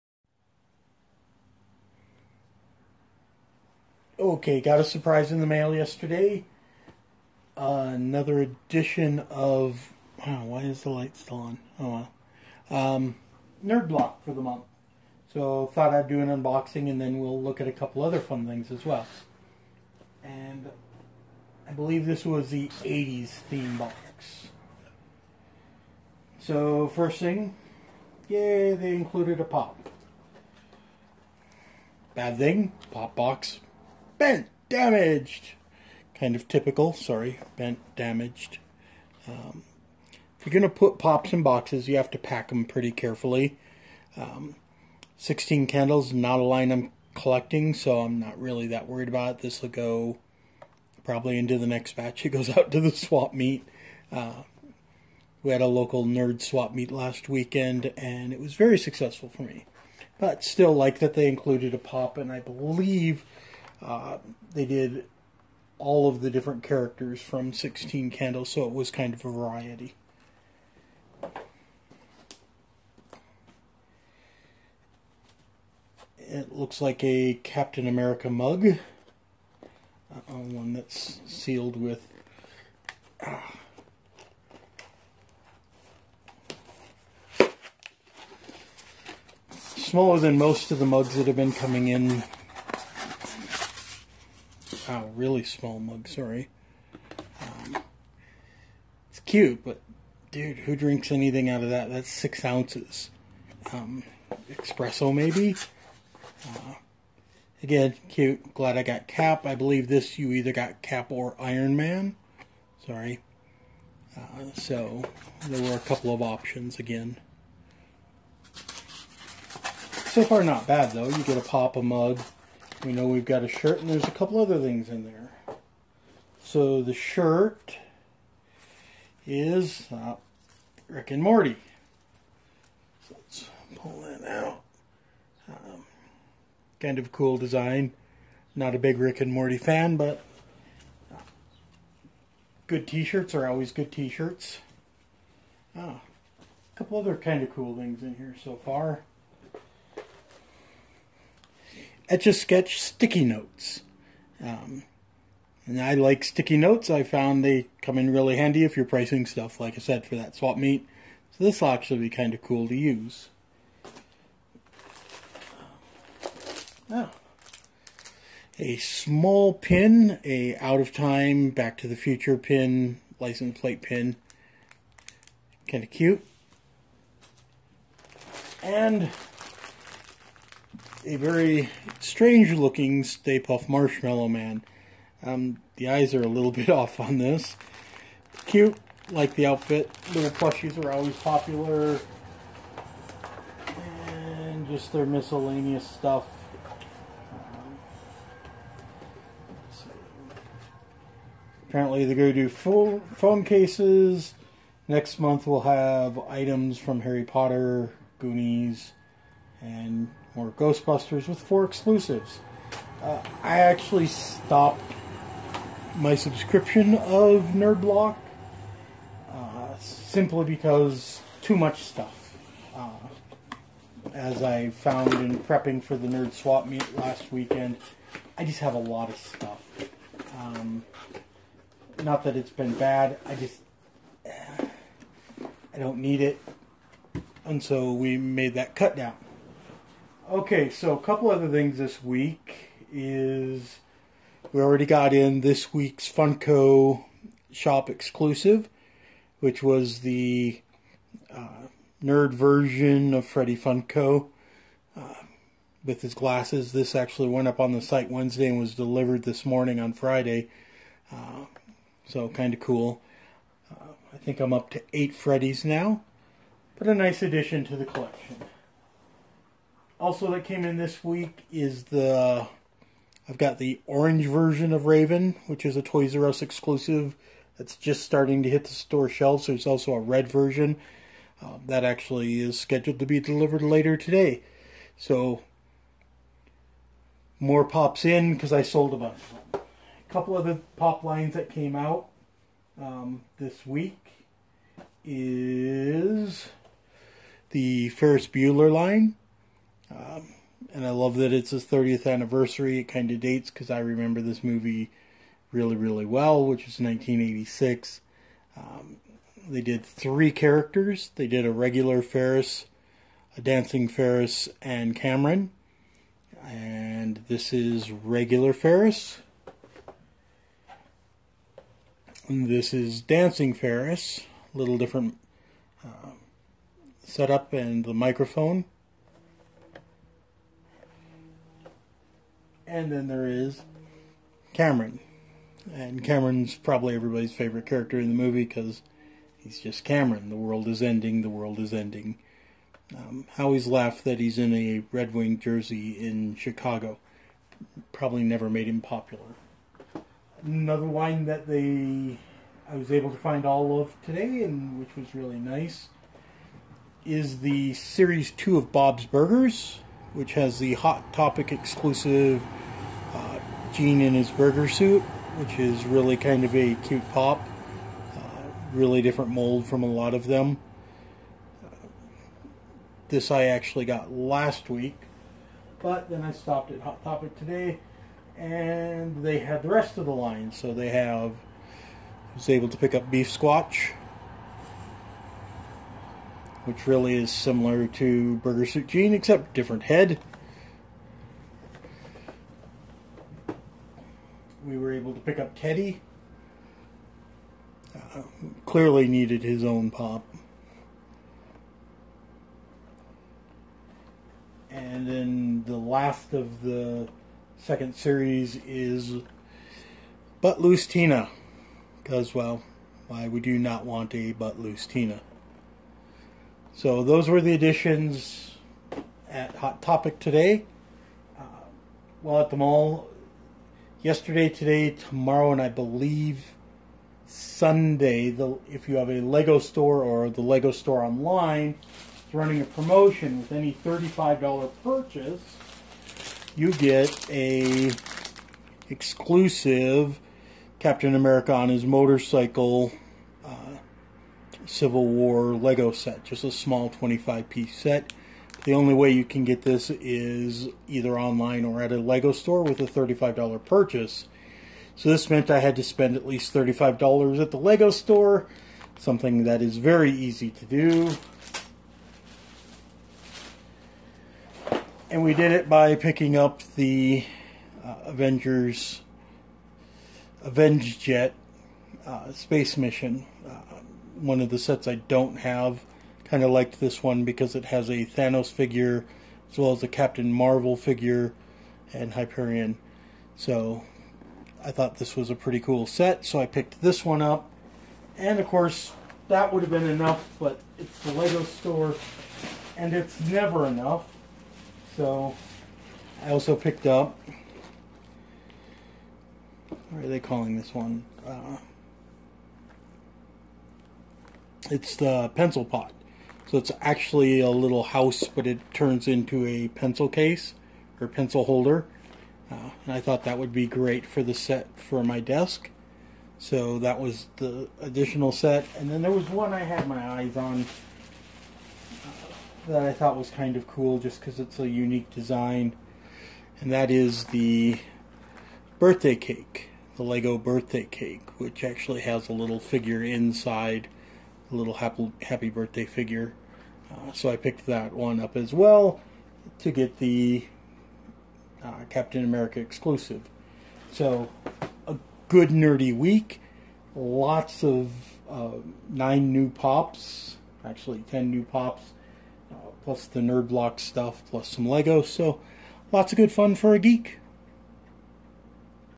May 2016 Nerd Block (80's theme) Unboxing & more
A look at the May Nerd Block, with the theme of 80's, as well as a look at what Funko Pops and Legos I picked up this week.